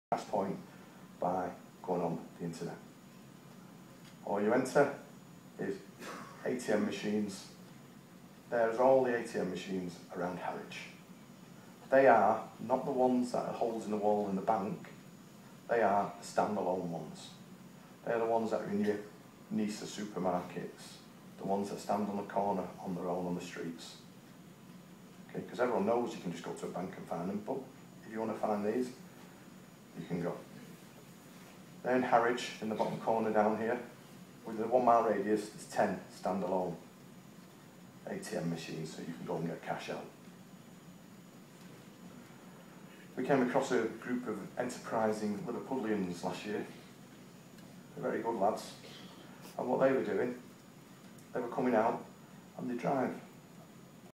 Kevin Rowley CLA
Kevin Rowley retired Detective and founder member of Cruise Lecturers Association presenting "Big Brother are you being followed"